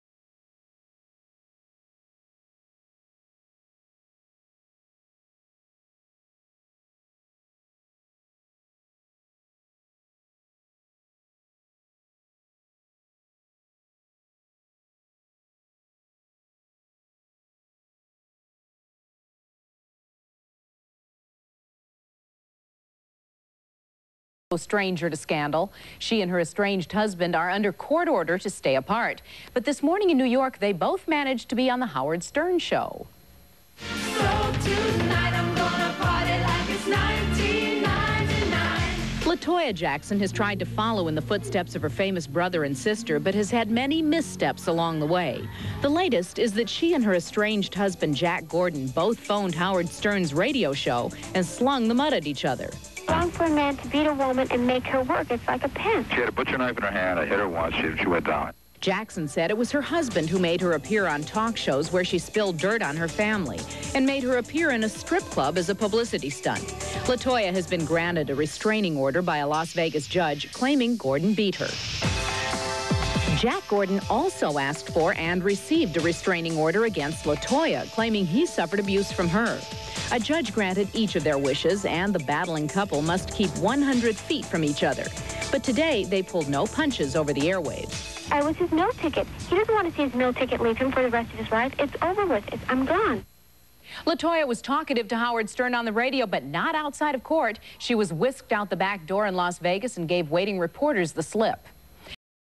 1996 Latoya Jackson Talks Jack Gordon's Abuse to Make Money Off Her!
In 1996, Latoya Jackson would escape Jack Gordon's abuse and accuse him of forcing her to pose nude in Playboy, perform at a strip club, and tell lies on the Jackson family.  Many OLD interviews of Latoya, circulating on YouTube, do not tell the FULL story of what happened to Latoya.  But in this video you will actually hear Jack Gordon say he HIT Latoya. Michael's mother, Katherine, believed that Latoya was under drugs when she was with Jack Gordon. If you view those interviews and look at her eyes, it's obvious something is wrong. Just compare to now and her old interviews before Jack. When Latoya escaped Jack, she been to communicate like the OLD Latoya.